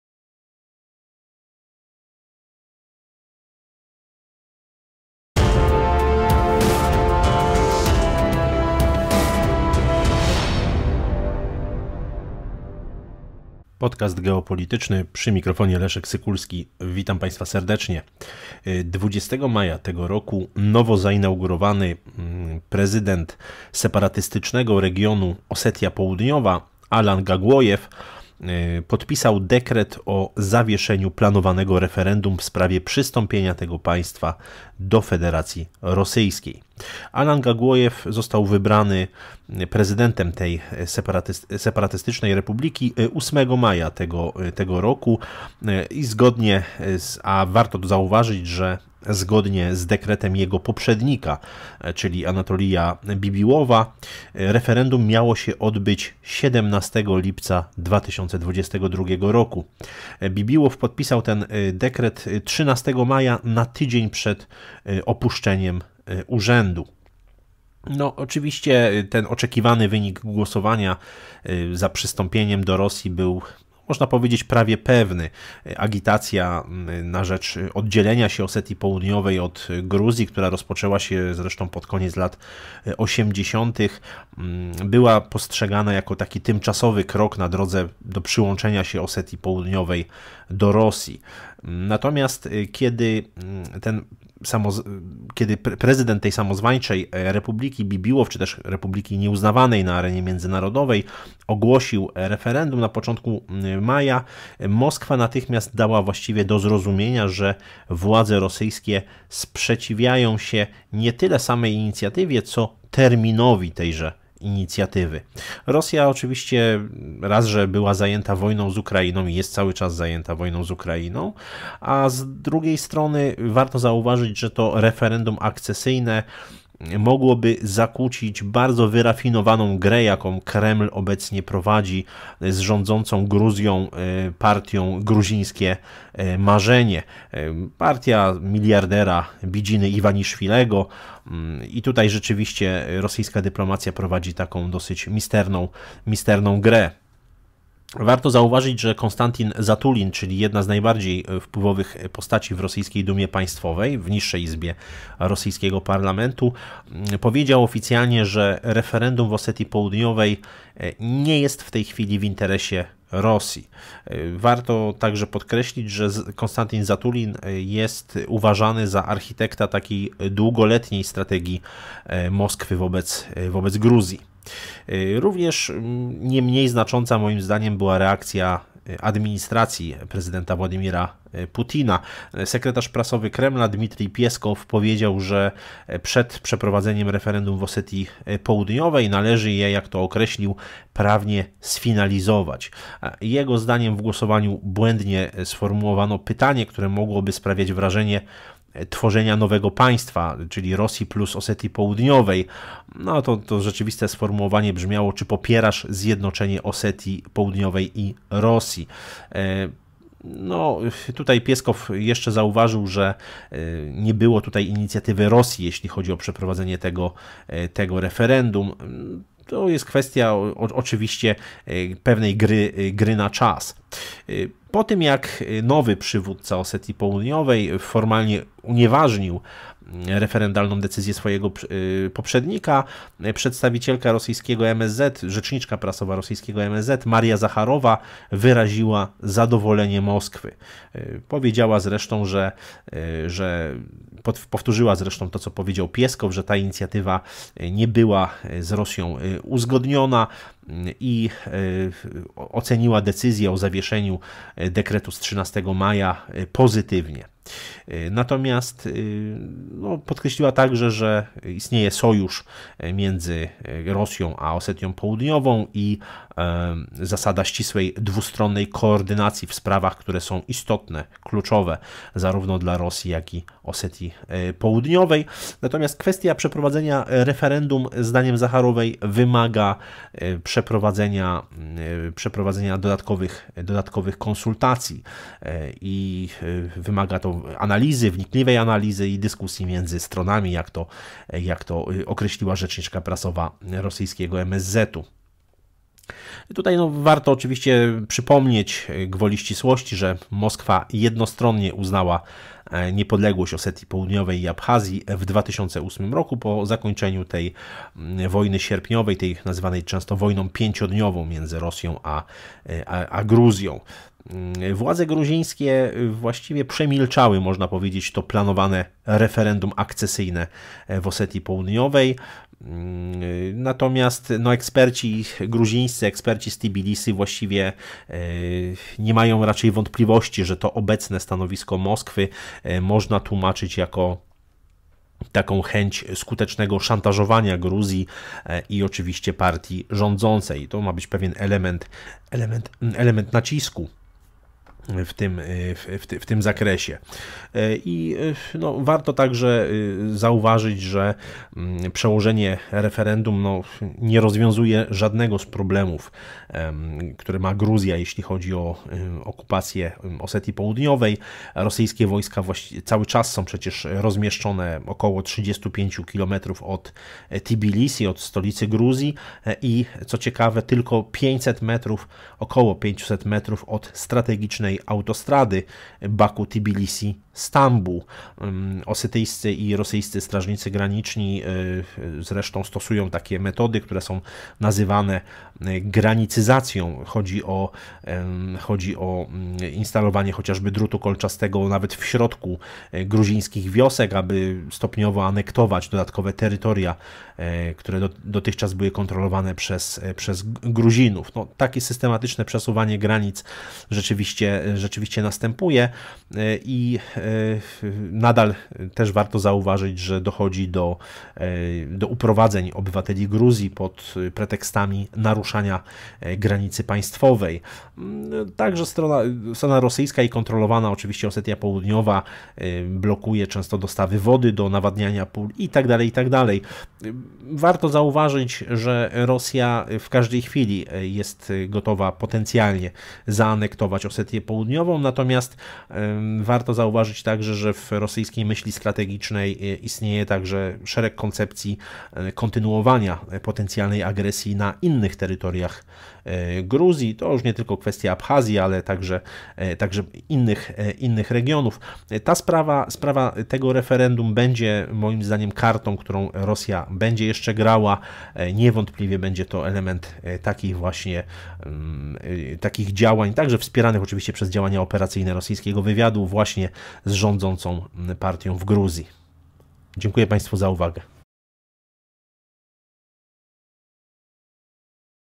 512. Moskwa odkłada w czasie referendum ws. przyłączenia się Osetii Południowej do Rosji
Podkast Geopolityczny Odc. 512 @SykulskiLeszek